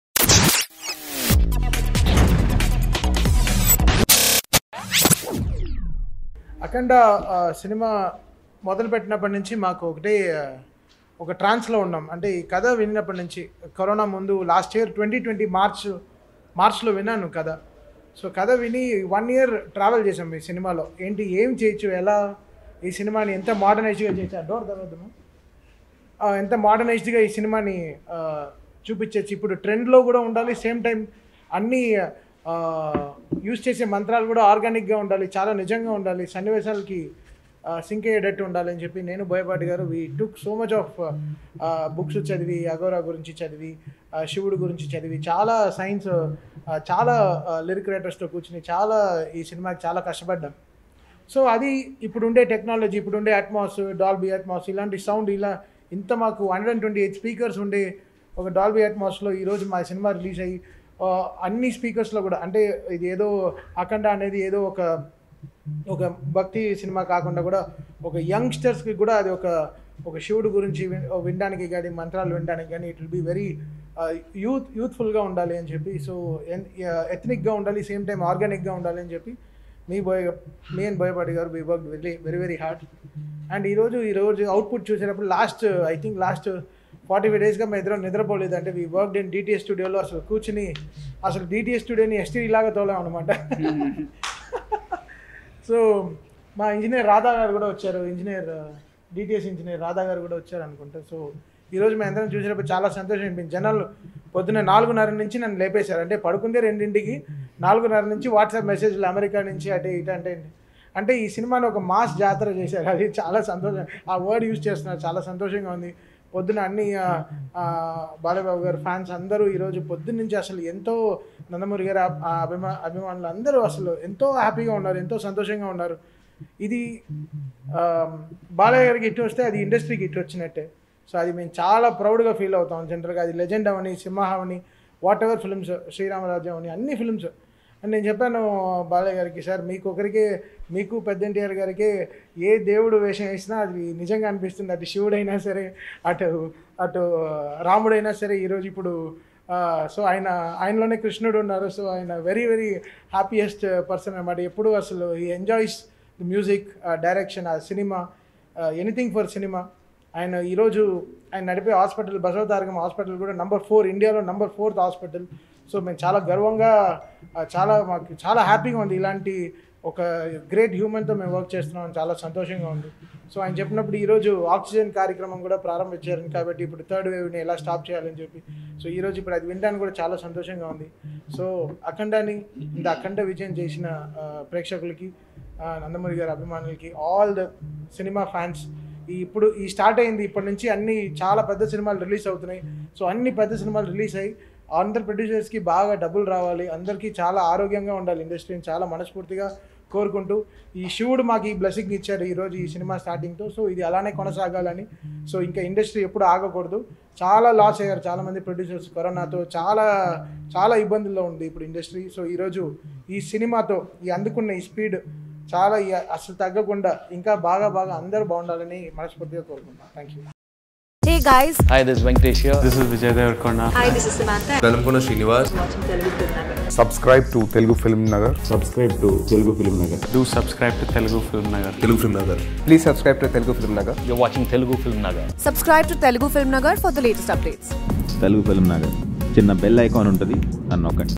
Thaman Superb Words About Balakrishna | Akhanda Movie Success Press Meet | Pragya | Boyapati Srinu
Thaman Superb Words About Balakrishna at Akhanda Movie Success Press Meet on Telugu FilmNagar. Akhanda Latest 2021 Telugu Movie ft. Nandamuri Balakrishna, Pragya Jaiswal, Poorna and Srikanth. Directed by Boyapati Srinu. Music by Thaman S. Produced by Miryala Ravinder Reddy under Dwaraka Creations banner. NandamuriBalakrishna​ BoyapatiSrinu​ Balakrishna PragyaJaiswal Poorna Srikanth ThamanS​ DwarakaCreations​

Akhanda Movie is out now in theaters with Sensational Blockbuster Hit Talk. Book your tickets and watch it today.